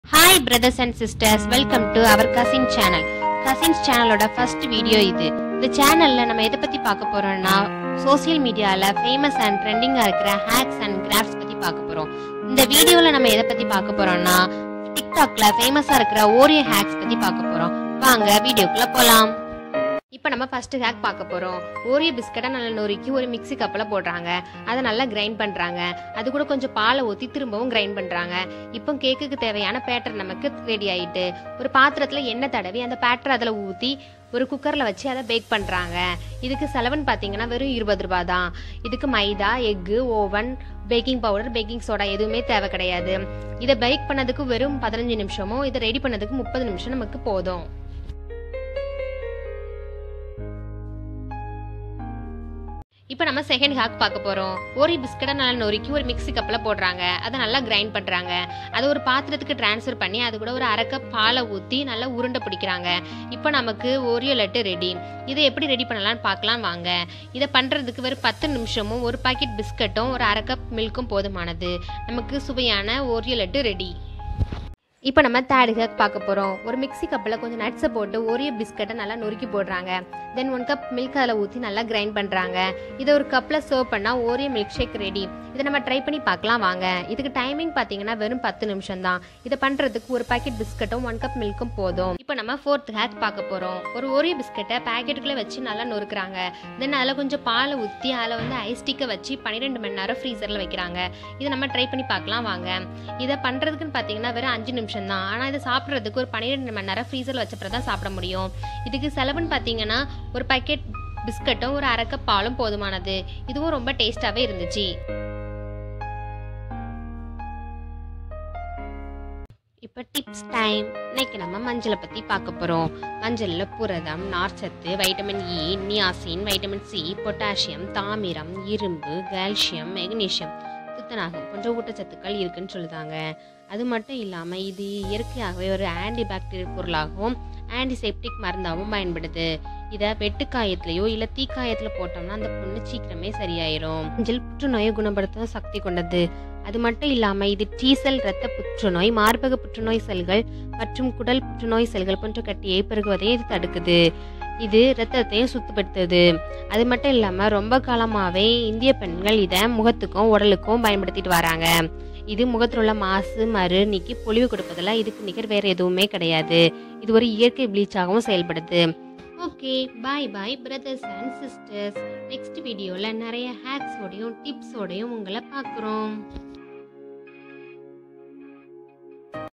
Tested TIKTOK famous OREO hacks 😋/ Tha Cousins (Tamil).
Hi brothers and sisters,
This is our first video . Based on TikTok oreo hacks.